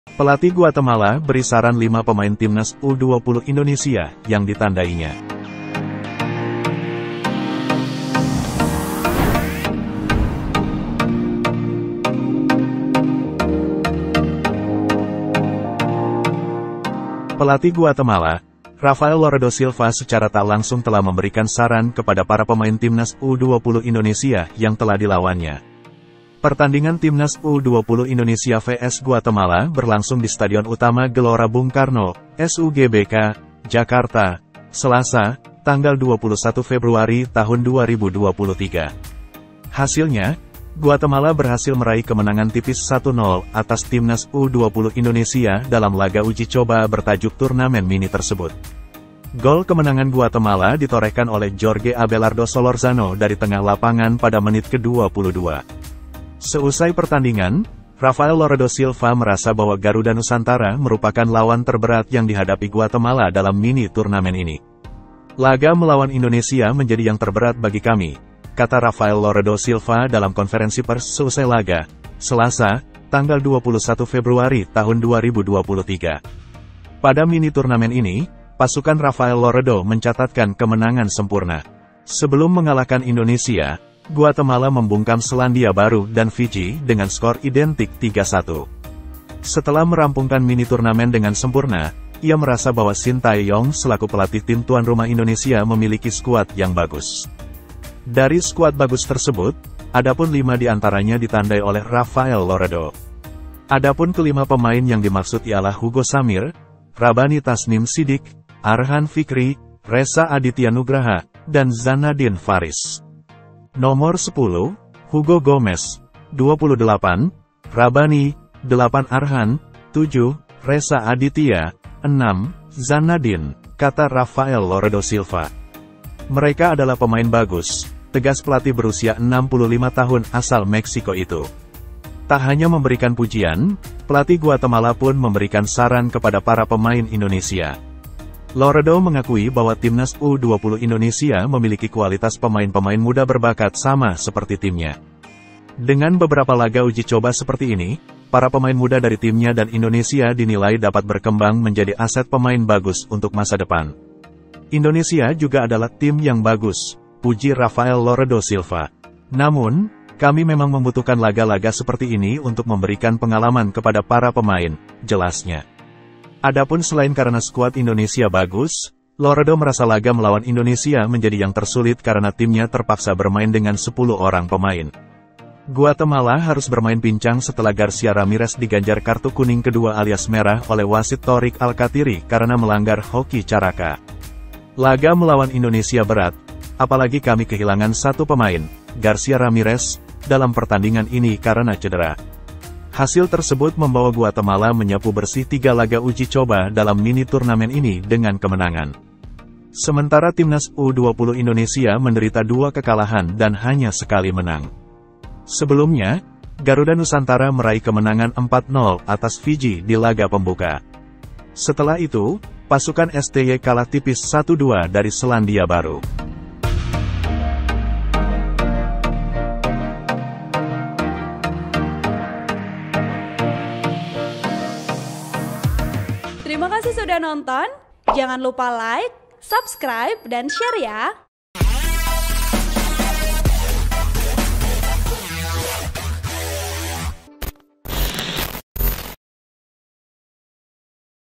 Pelatih Guatemala Beri Saran 5 Pemain Timnas U-20 Indonesia yang Ditandainya
Pelatih Guatemala, Rafael Loredo Silva secara tak langsung telah memberikan saran kepada para pemain timnas u-20 Indonesia yang telah dilawannya.
Pertandingan timnas u-20 Indonesia vs Guatemala berlangsung di Stadion Utama Gelora Bung Karno (SUGBK), Jakarta, Selasa (21/2/2023).
Hasilnya, Guatemala berhasil meraih kemenangan tipis 1-0 atas timnas U-20 Indonesia dalam laga uji coba bertajuk turnamen mini tersebut.
Gol kemenangan Guatemala ditorehkan oleh Jorge Abelardo Solorzano dari tengah lapangan pada menit ke-22.

(IAG)